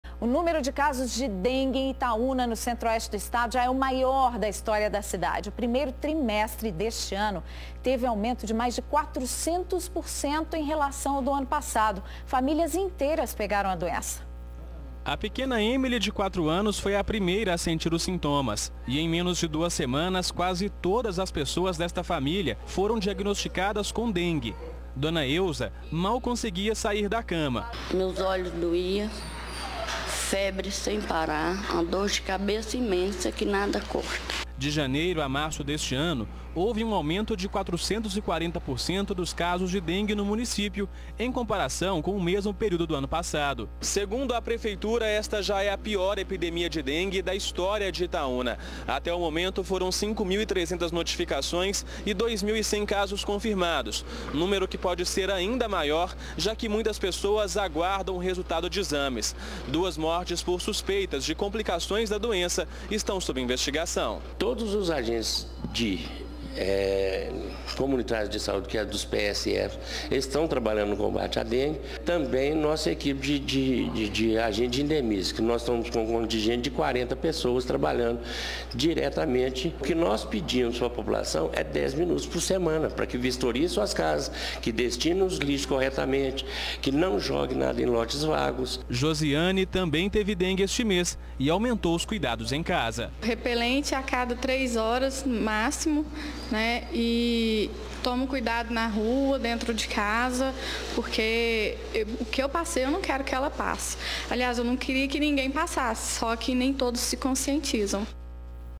Itaúna registra a maior epidemia de dengue
A cidade de Itaúna, no centro- oeste do estado, registrou a maior epidemia de dengue na história da cidade.